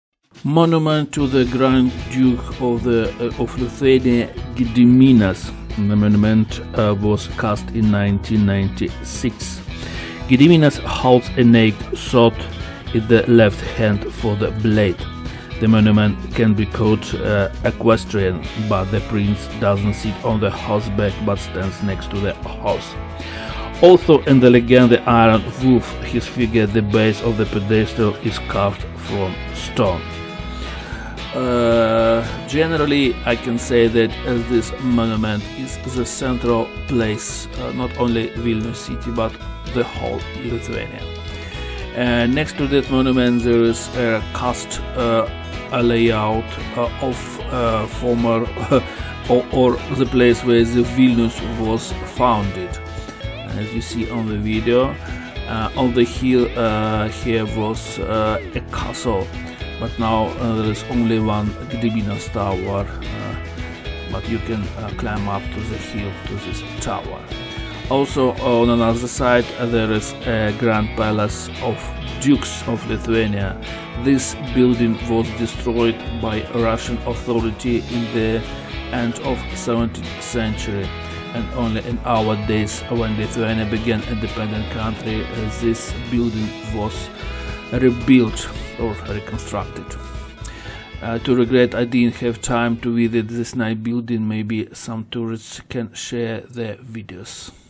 Lithuania; Vilnius city; Monument to Gediminas; Palace of the Dukes of Lithuania Photo; Video
Lithuania; Vilnius city; Monument to Gediminas - a monument to one of the founders of the city; Palace of the Dukes of Lithuania - a monument of Lithuanian statehood; It was destroyed by the Imperial Russian authorities in the 17th century and restored today; Photo; Video